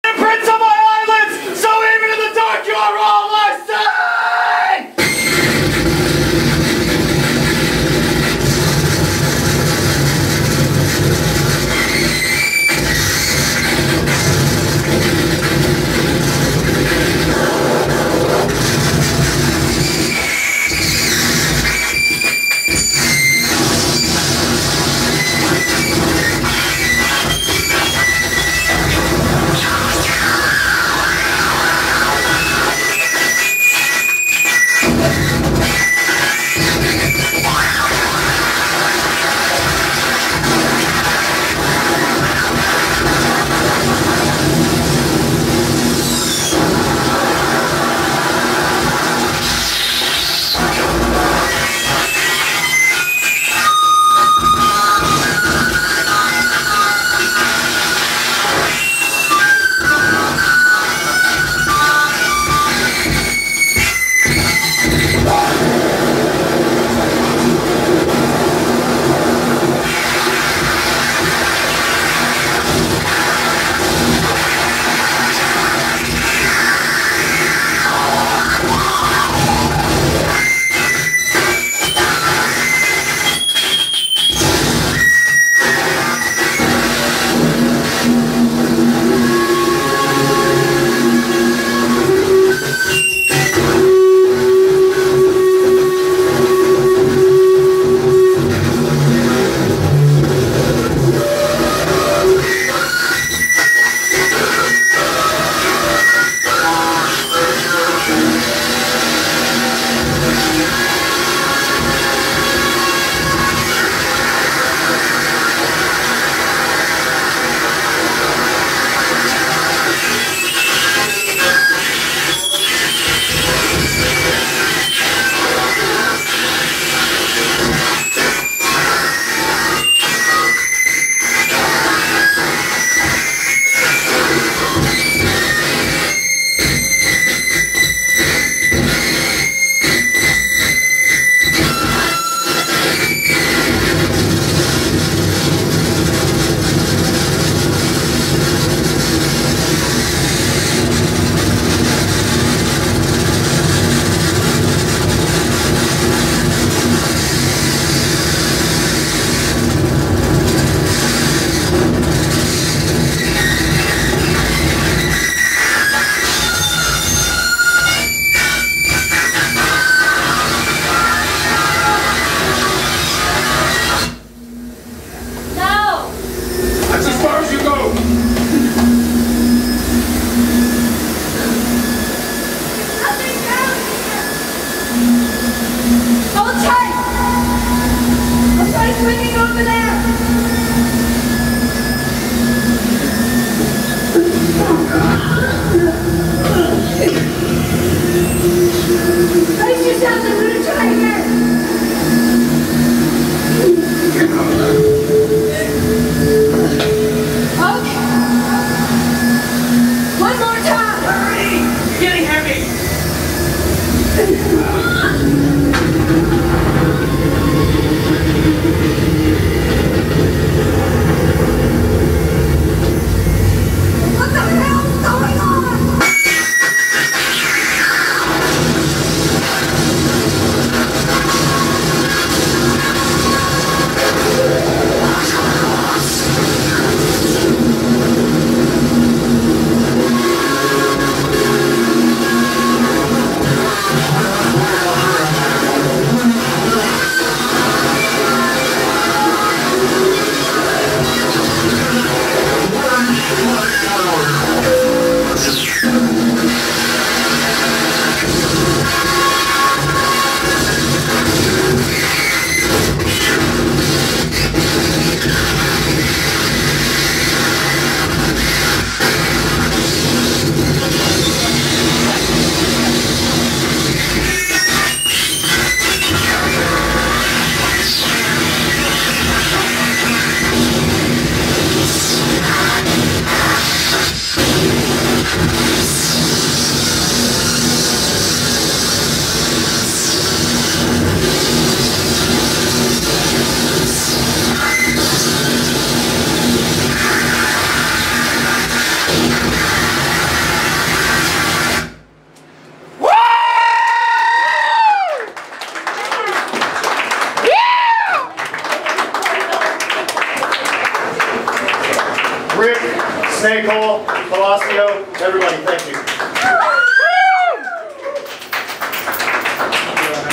Human Fluid Rot @ Palacio De Los Jugos 10/3/15
Alternative MIA & Palacio De Los Jugos presents

SOUND IS WEAPON v2
Loud noisy gathering of freaks
featuring performances by:
HOROSCOPE (ny)
SHARLYN EVERTSZ
HUMAN FLUID ROT
ONSET
GLASS ARCADE
SNAKEHOLE
GWAND
CITY MEDICINE

256 NW 53rd st